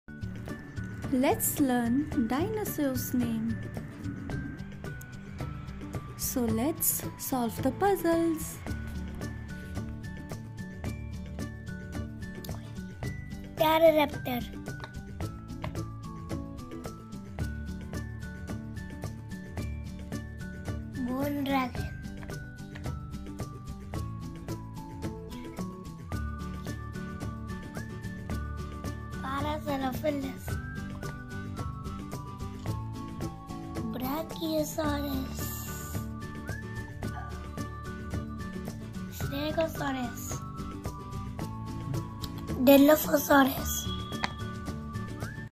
Can you solve the Dino Puzzle in just 60 seconds? Jurassic Jumble challenge!
Dive into a thrilling race against the clock with our "Jurassic Jumble" challenge! This fast-paced, adrenaline-pumping experience tests your wits and speed as you attempt to piece together a prehistoric puzzle. As the seconds tick away, you'll be surrounded by iconic dinosaurs, from the mighty T-Rex to the graceful Pterodactyl. But beware, with each passing moment, the challenge intensifies! Whether you're a seasoned puzzle enthusiast or a dino-fanatic, this 60-second showdown promises excitement, suspense, and a whole lot of roaring fun. So, gear up, set your timer, and let's see if you can conquer the Dino Puzzle before time runs out!